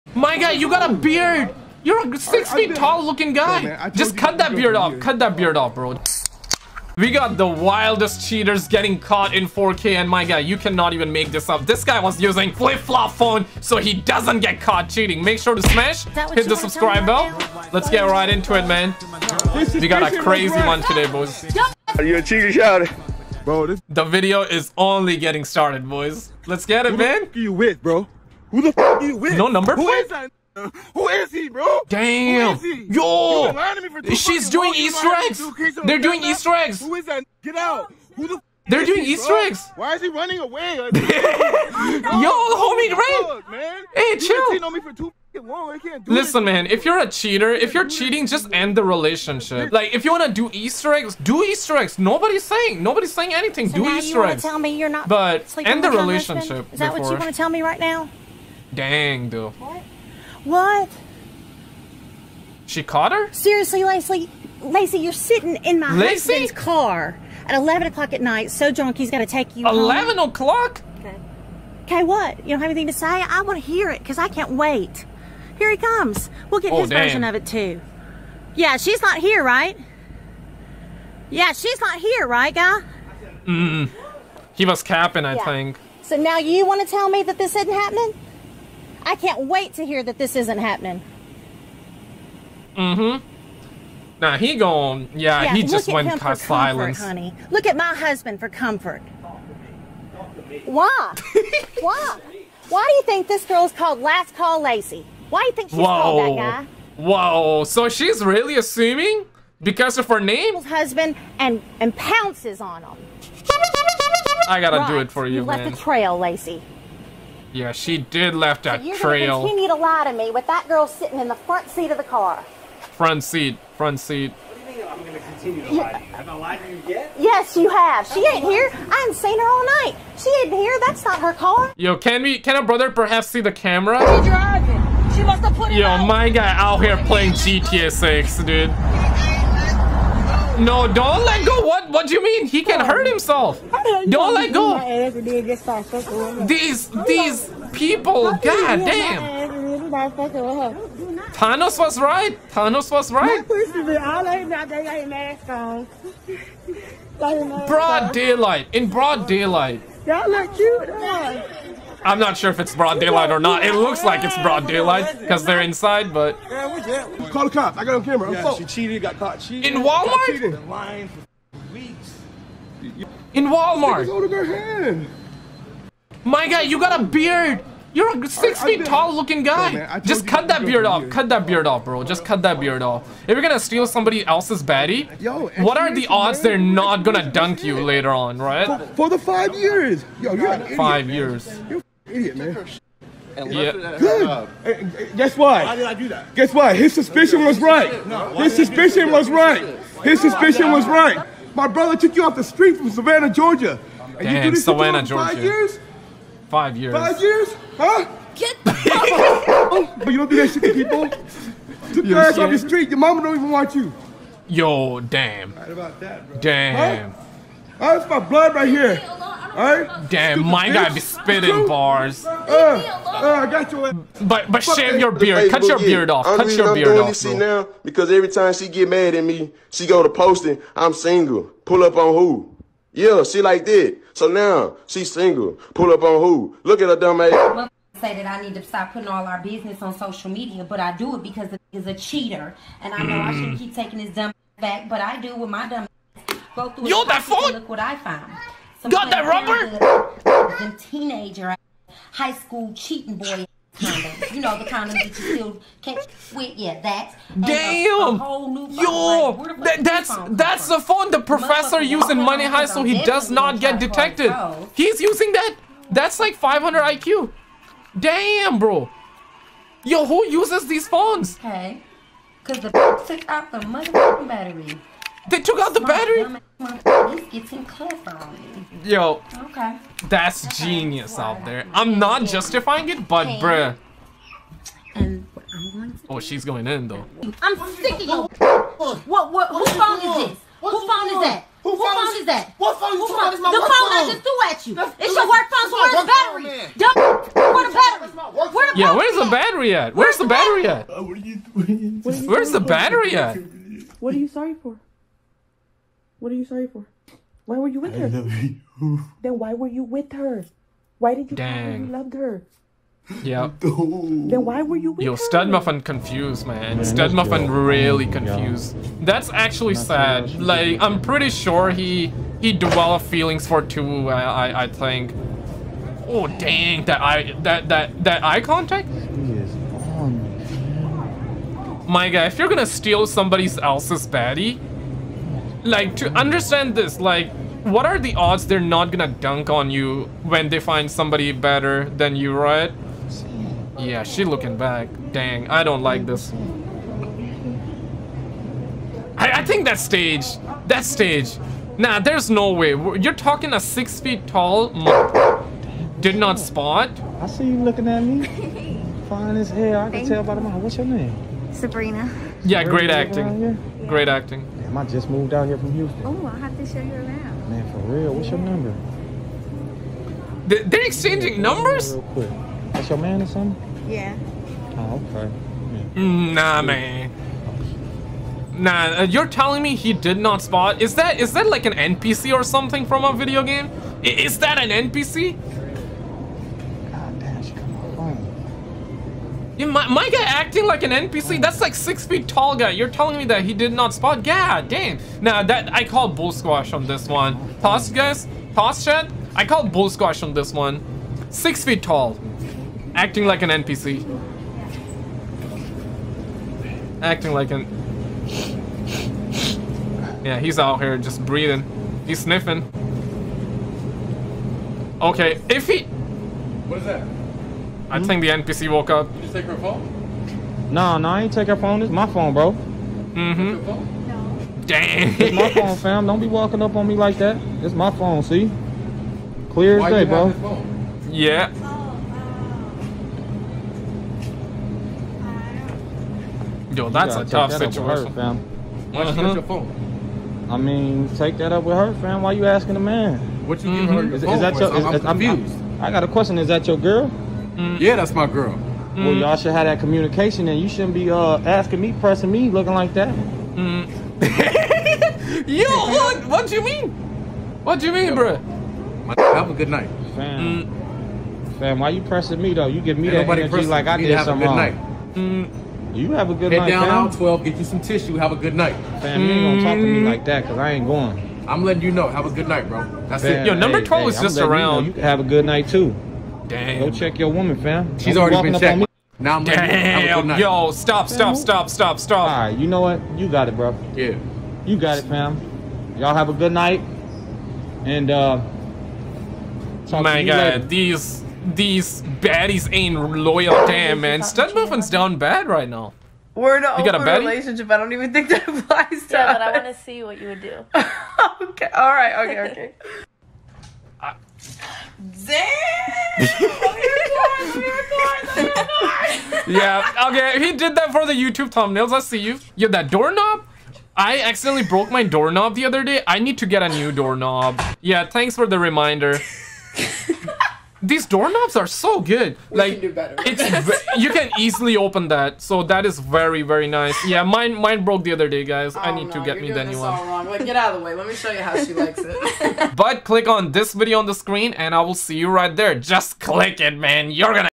Girlfriend Caught CHEATING Red Handed 😨 (Try Not To Laugh Dank Memes)
Cheaters Caught RED HANDED (Try Not To Laugh Dank Memes)

This is a Cheaters Caught series where we will be watching Reddit, YouTube & Tik Tok videos of people getting caught cheating on camera. Consider this as a funny meme try not to laugh challenge type series but where people's girlfriend is getting caught cheating on camera. Hope you all enjoy these funny tiktok meme & cheaters caught videos.